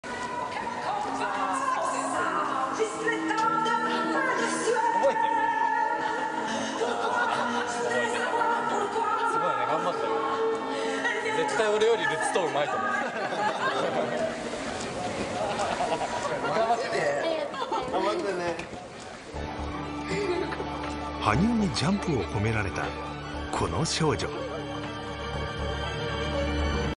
WTT 2015 - Yuzuru Hanyu chats with Yuna Aoki
It's so cute how Yuna was giggling & fixing her hair after Yuzu left.  You can tell that she was totally fangirling 😍